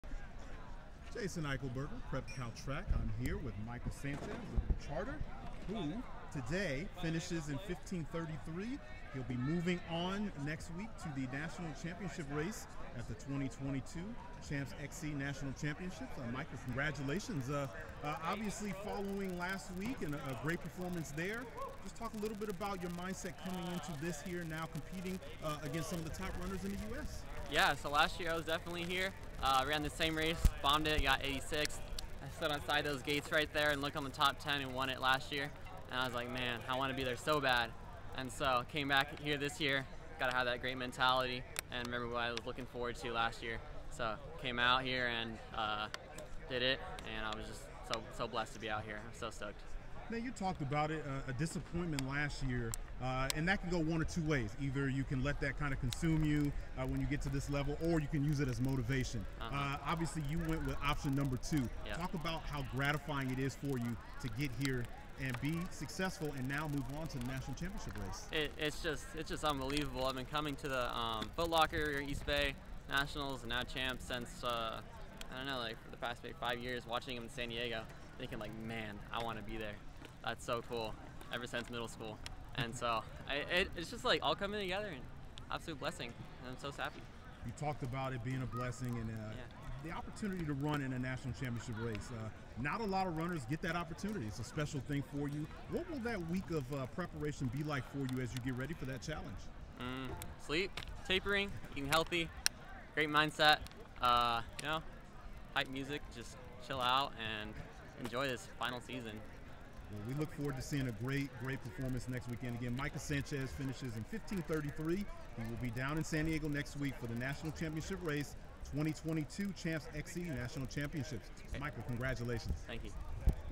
Liberty Charter's Micah Sanchez places 5th at Champs Sports West Regional!
Liberty Charter's Micah Sanchez, based out of San Diego County, fulfilled a major goal by qualifying for his first national-level race. Micah placed 5th at the Champs Sports West Regional meet at Mt. San Antonio College to qualify for next weekend's Champs Sports Nationals meet in San Diego. Here's Micah's post-race interview with Jason Eichelberger.